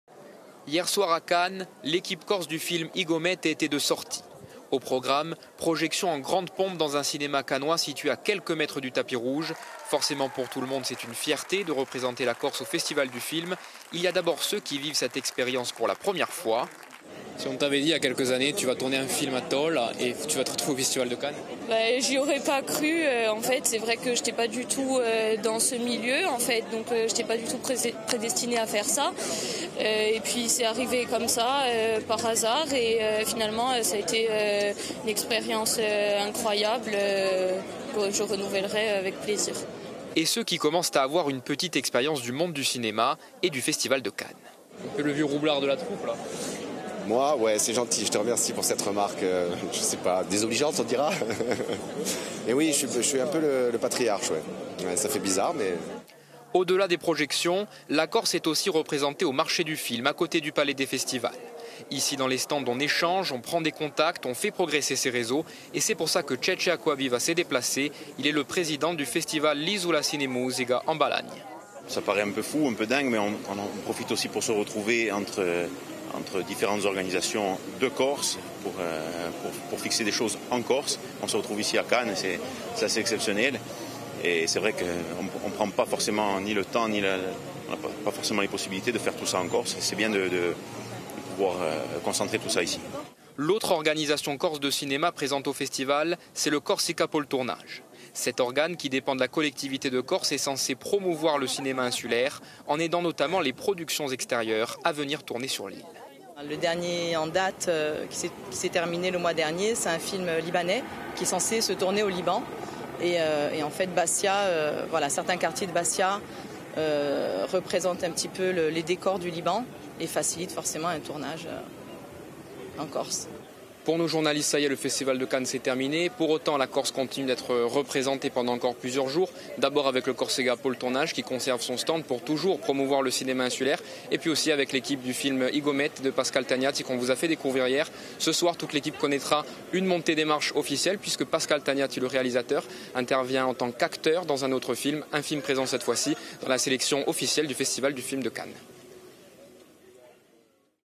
La Corse présente à la 74ème édition du festival du film de Cannes !
Intervenants -
Livia Vittini
Comédienne "I Comete"

Cédric Appietto
Comédien "I Comete"

Ceccè Acquaviva
Président du festival "Lisula Ciné Musica"

Nathalie Gresse
Corsica pôle tournage

Equipe -
Jean-André Marchiani
Guillaume Leonetti
Bertrand Pasbeq